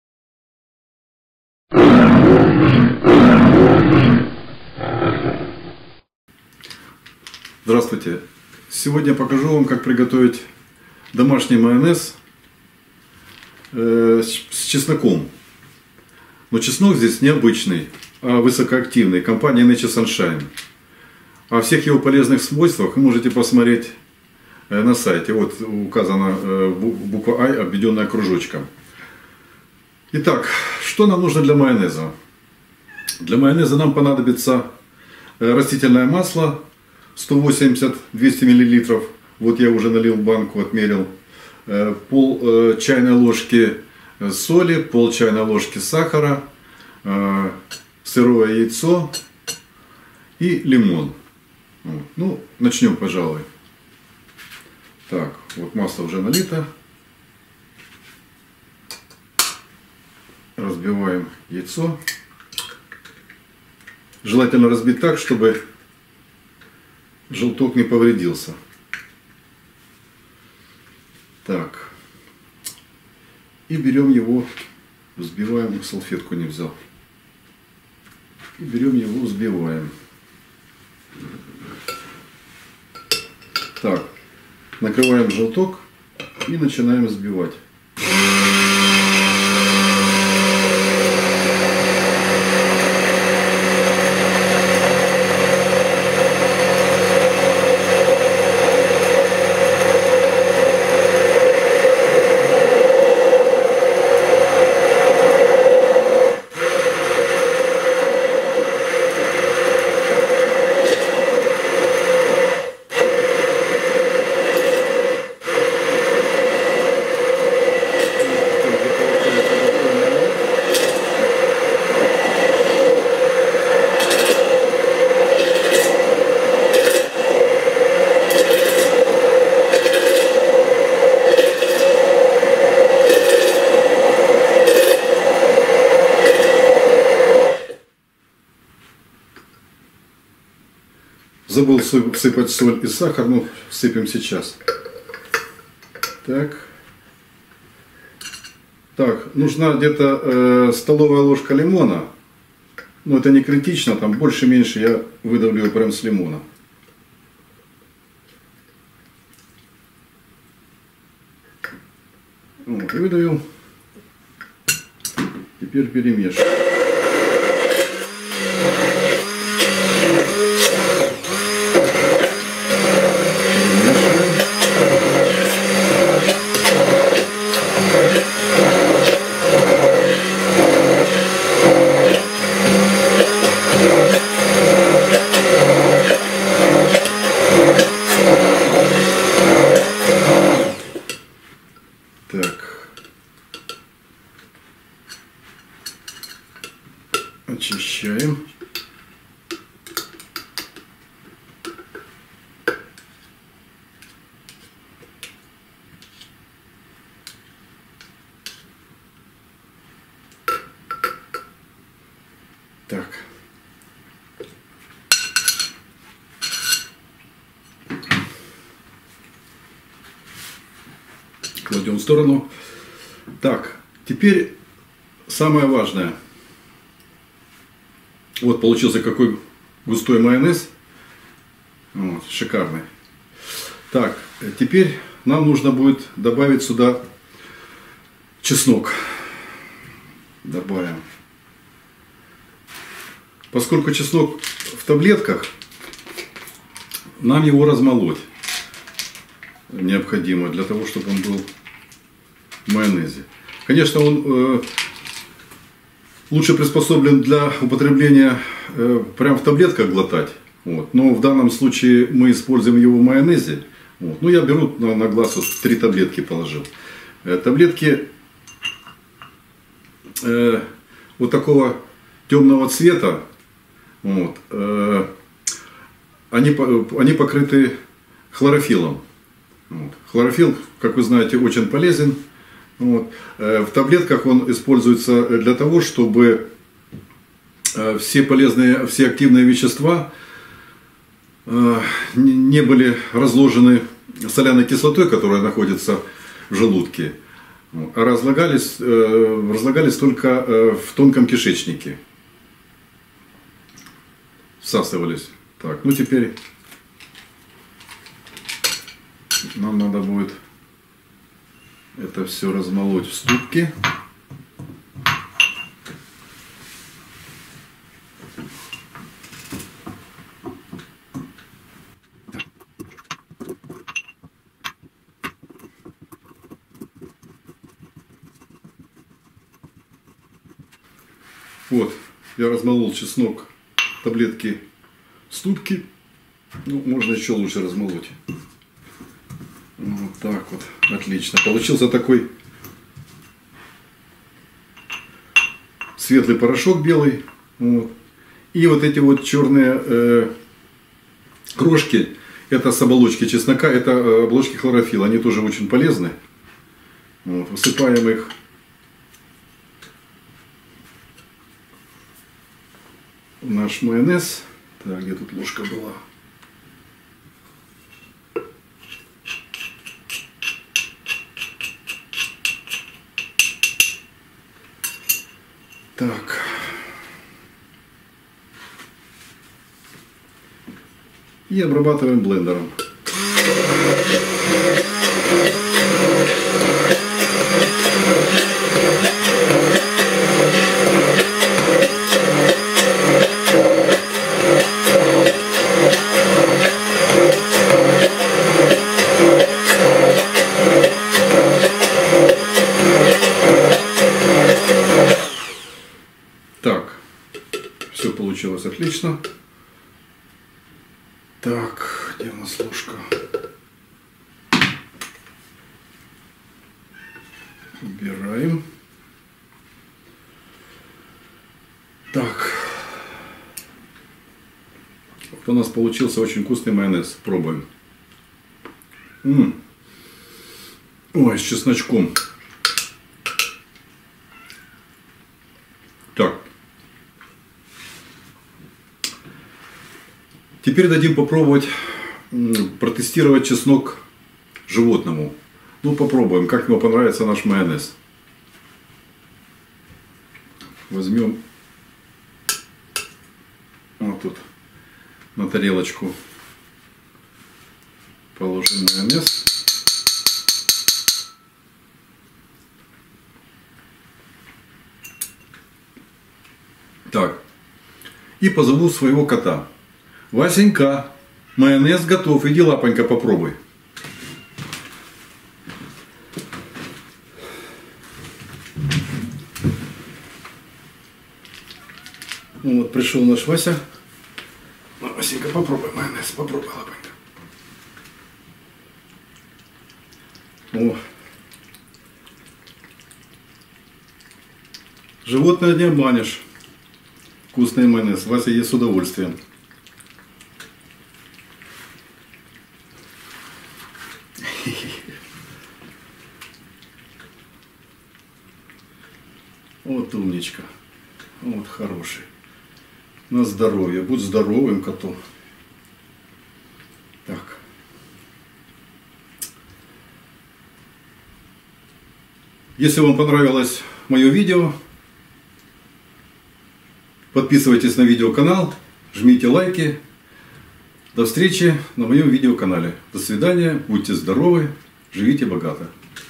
Майонез с чесноком NSP - рецепт домашнего майонеза в домашних условиях
Ингредиенты майонез с чесноком:
• Масло подсолнечное рафинированное - 180-200 мл;
• Яйцо куриное (сырое) - 1 шт;
• Соль - 0,5  чайной ложки;
• Сахар - 0,5  чайной ложки;
• Сок лимонный - 1  столовая ложка;

Это произведение доступно по лицензии Creative Commons «Attribution» («Атрибуция») 4.0 Всемирная.